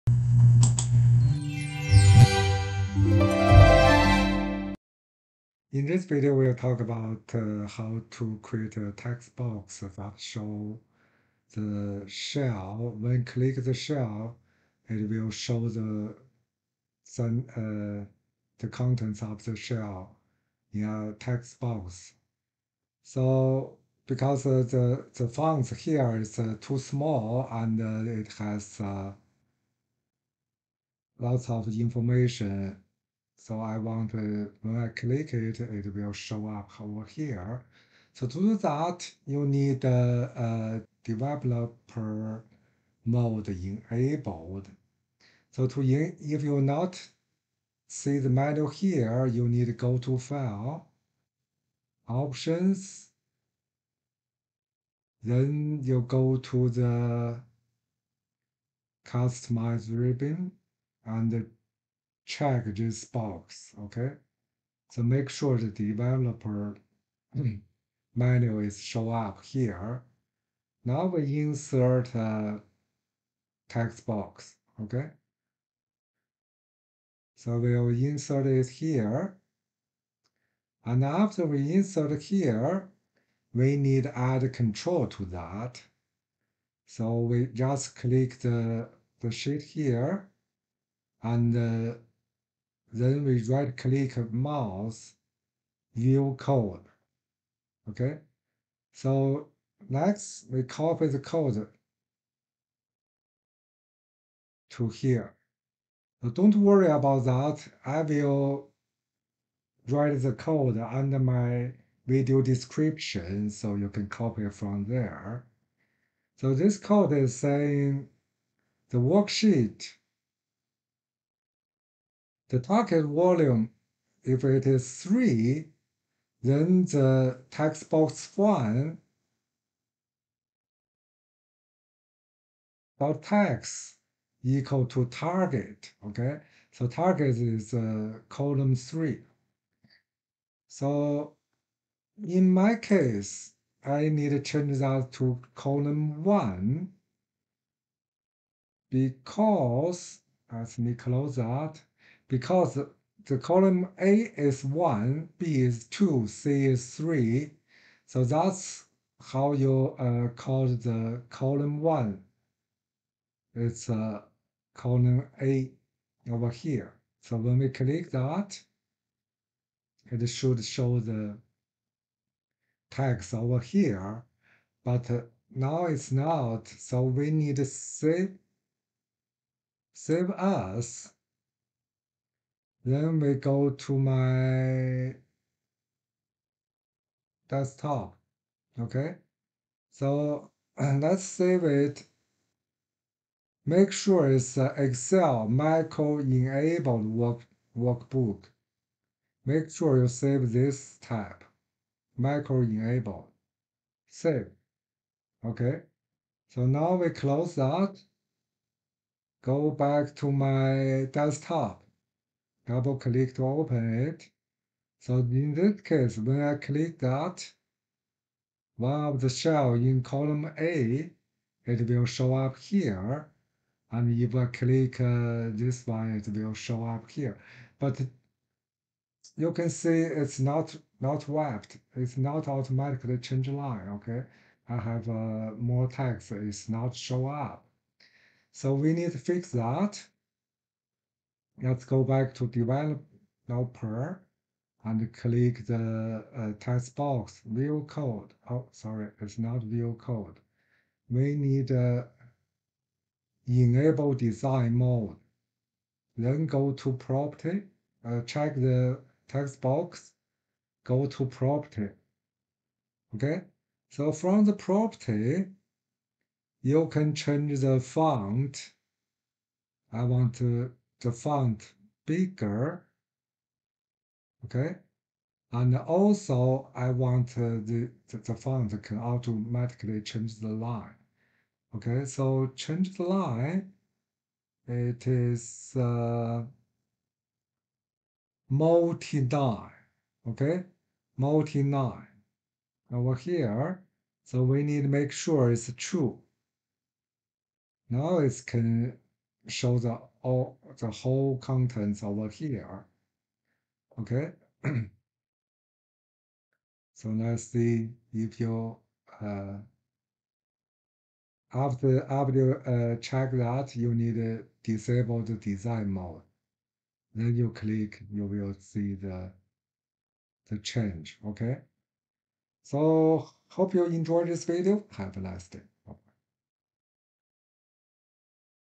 MS Excel show click cell text in text box 1
Is it possible to link multiple cells to the same text box so clicking different cells displays different text?
Dynamically display cell range contents in MS Excel text box.

You need past the code to your code:

Private Sub Worksheet_SelectionChange(ByVal Target As Range)
 If Target.Column = 1 Then ActiveSheet.TextBox1.Text = Target else ActiveSheet.TextBox1.Text = ""
End Sub

Then you can change MS Excel text box font and font size from font property.

If you want MS Excel text box wrapping text (Word Wrap). then you need make sure change it's Multiline property to True